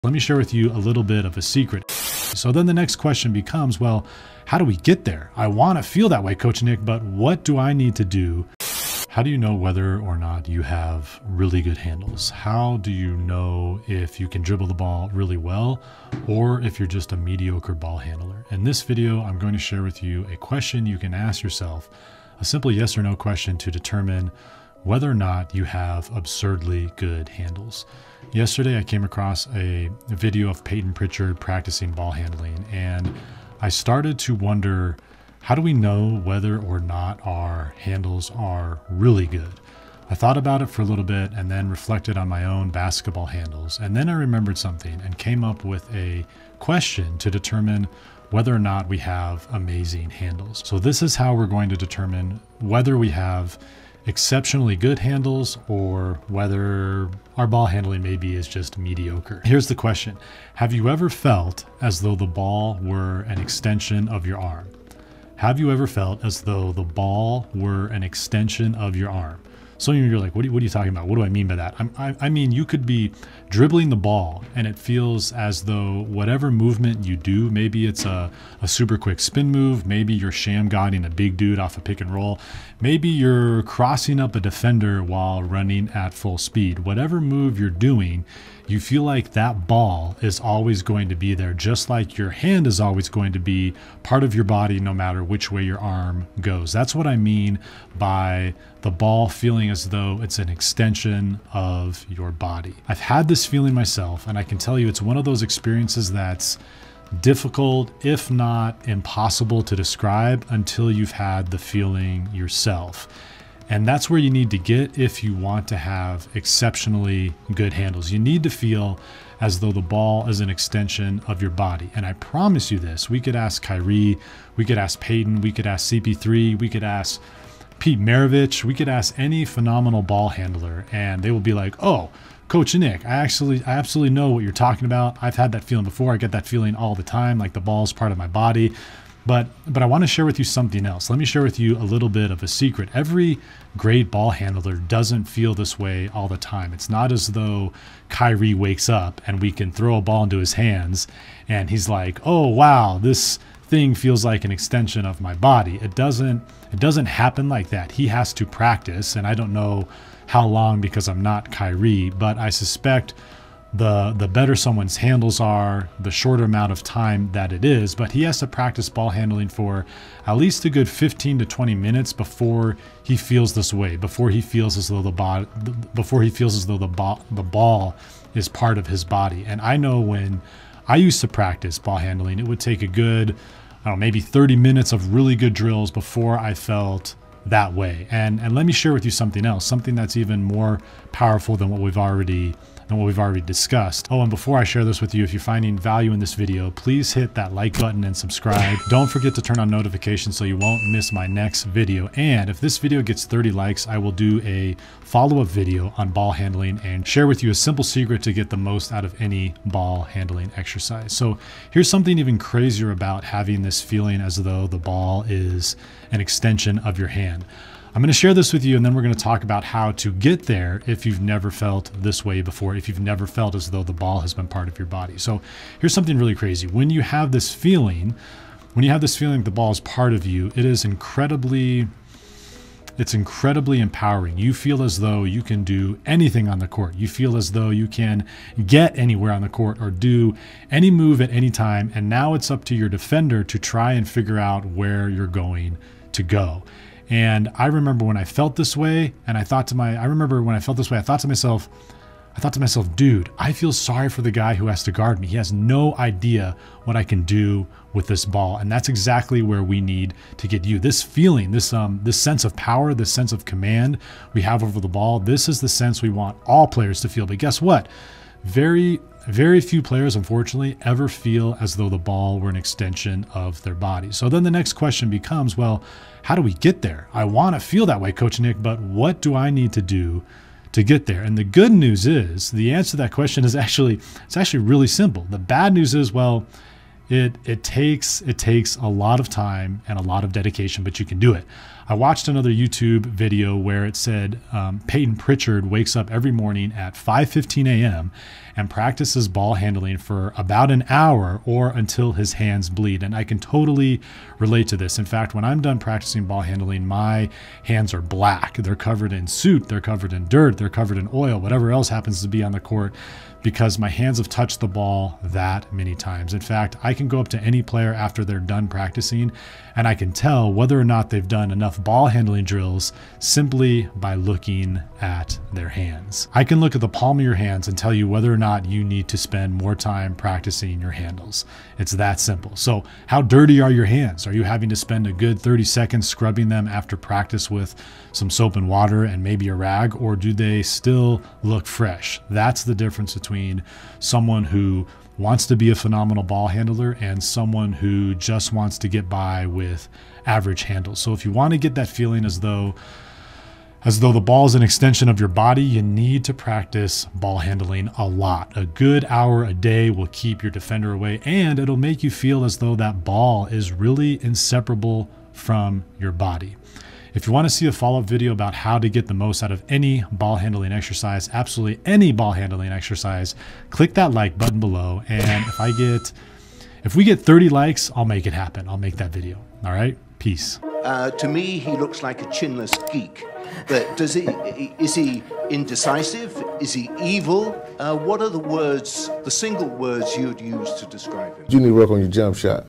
YOU GOT GOOD HANDLES? | How to know if you're basketball handles are good and what to do about it.
How good are your basketball handles?  Can you dribble anywhere at anytime and w/ any move?  Let's talk.